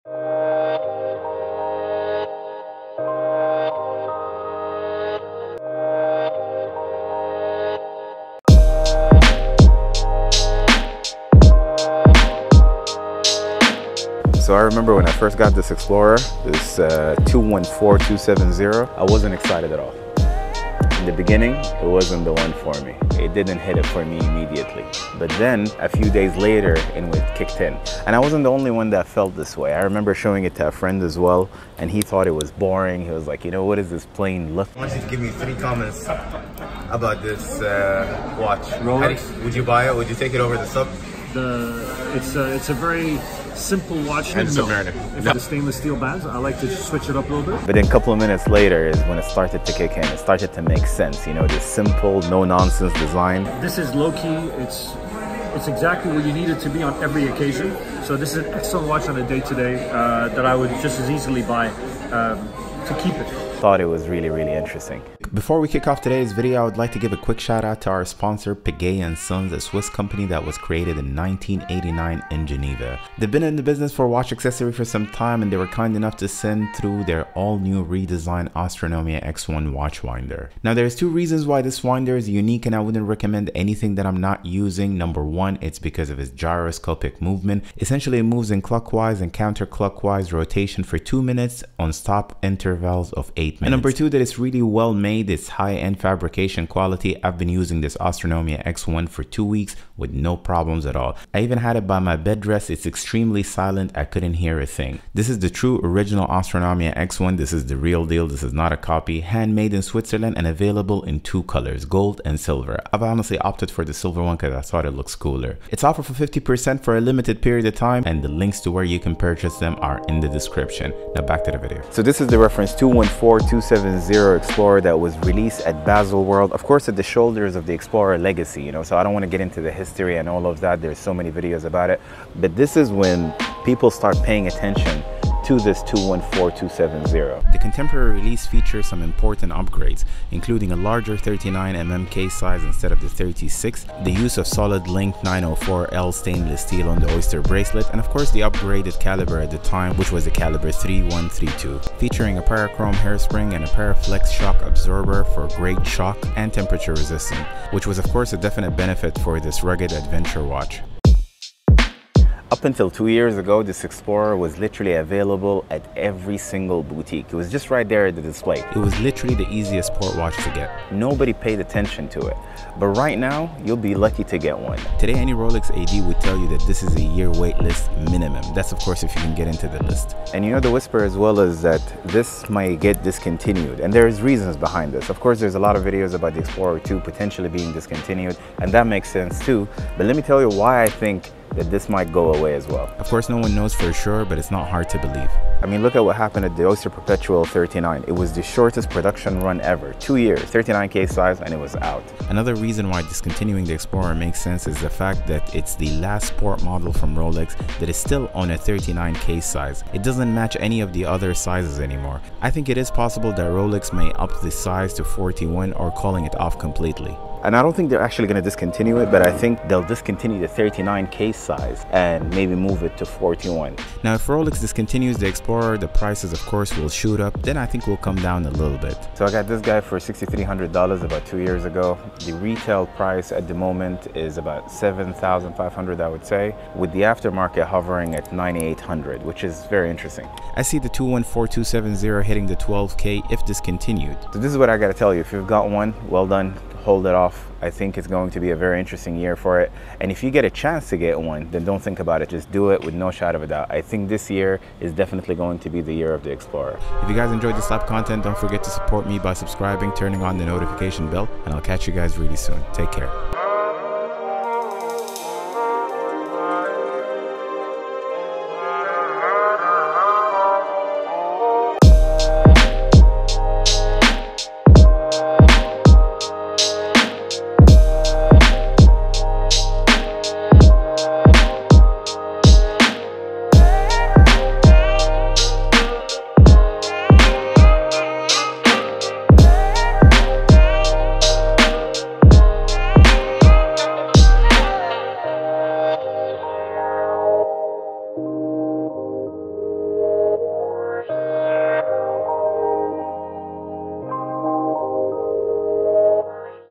Buy This Rolex Now While You Still Can! Soon To Be the Hottest Rolex!
Be it in the greater world of watches or just within the context of Rolex, by far my favourite Rolex, the Rolex Explorer 214270, i am suggesting buy this Rolex now while you still can! soon To be the hottest rolex!

0:00 Intro
0:15 When I first Got the Explorer
0:33 What My Friend Thought About The Explorer
01:05 Piguet&Son - Sponsored Content
02:02 The Rolex Explorer 214270
02:37 Rolex Explorer availability
03:00 Will Rolex discontinue the Explorer in 2021?
03:47 Pricing & after market price